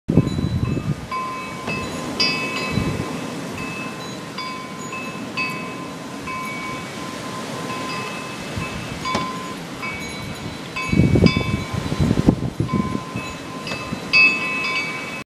A windy afternoon in SW Sask.
Short video of the gusting wind here in Morse, Sk. 5 Dec. 2012
TWN says these gusts are 55km, I beg to differ.